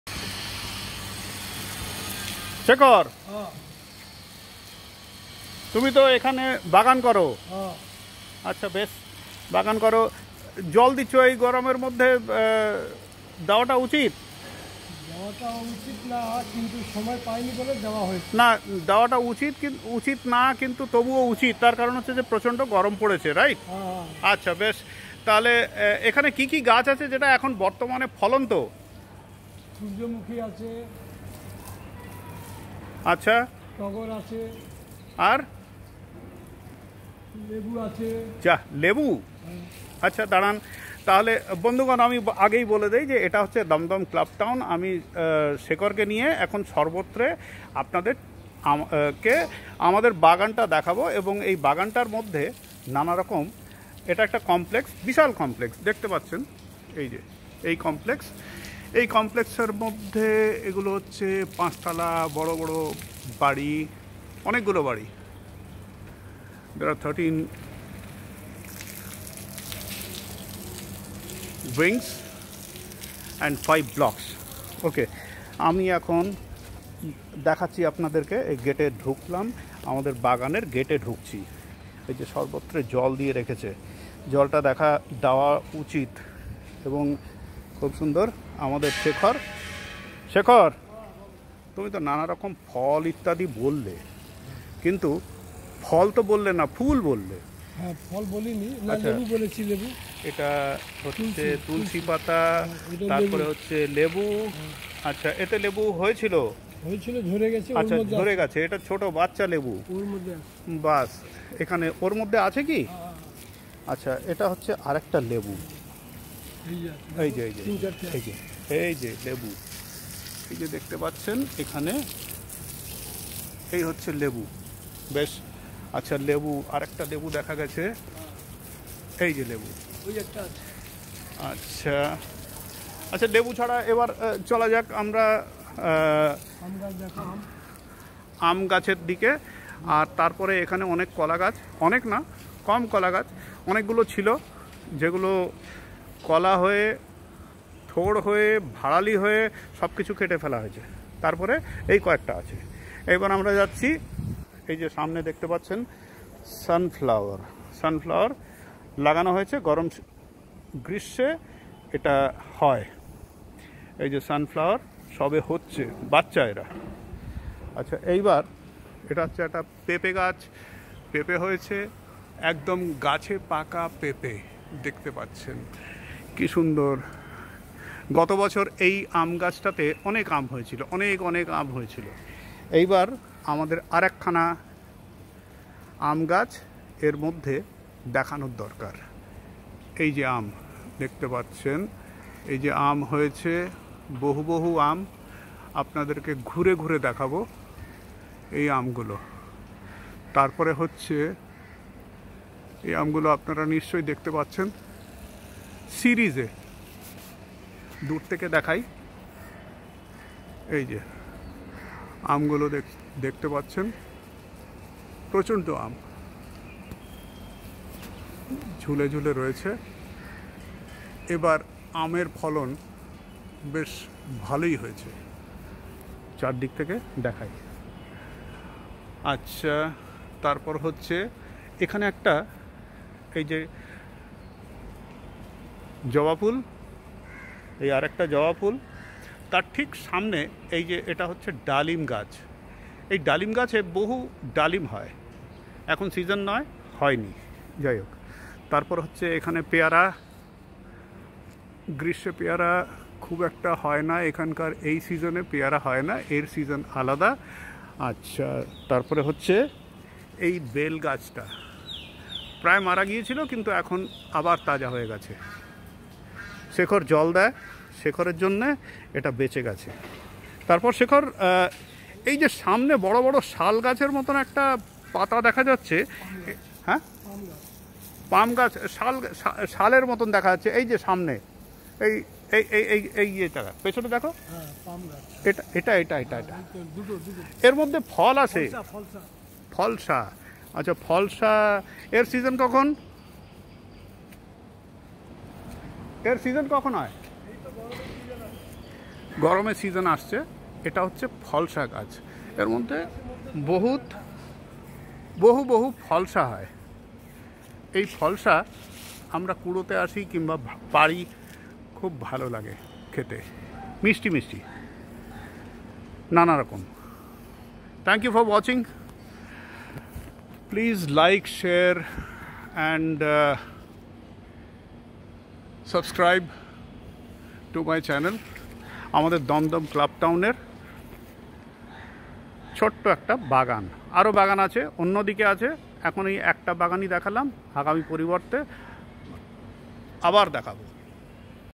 Home Dumdum Clubtown Estate Cool Summers Wet & Green Garden Oxygen
II Dumdum Club Town II Cool Summers @ 40 O’ C II Keep Your Garden Wet & Green II
II Nature Has Its Ways II   Look Green Feel Good II
Here we take you for a short journey into the gardens of Dumdum Club Town Estate Complex Garden. All green, healthy & young looking plants, flowers & fruits.

Summers are not that bad if you know how to keep it cool. Keep your garden green, healthy growing & quenched. Nature has its ways to keep you safe from the heat wave, cool air & provides with abundant oxygen.

Equipment’s used
Samsung 9+ mobile phone

An amateurs' attempt on YouTube.